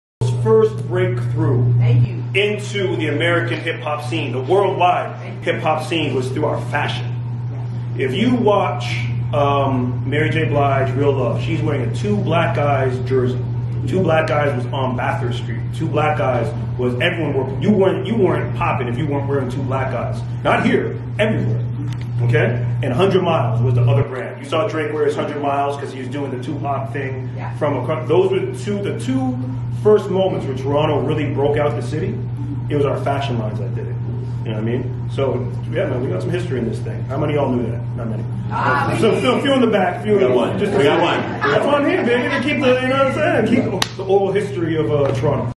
Director X | Spitting Facts | History Of | 100 Miles Brand | 100 Miles Sweater
100milesbrand @directorx wearing our "100 Miles" sweater spitting facts about the history of @100MilesBrand and Toronto's impact on the culture.
Special Thanks- @directorx